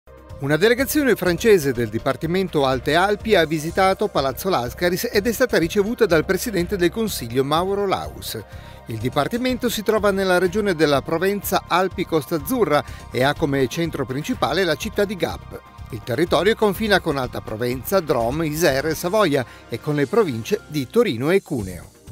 Delegazione francese a Palazzo Lascaris
Una delegazione francese del Dipartimento Alte Alpi ha visitato Palazzo Lascaris ed è stata ricevuta dal presidente del Consiglio, Mauro Laus. Il dipartimento si trova nella  regione della Provenza-Alpi-Costa Azzurra e ha come centro principale la città di Gap. Il territorio confina con Alta Provenza, Drome, Isere e Savoia, e con le province di Torino e Cuneo.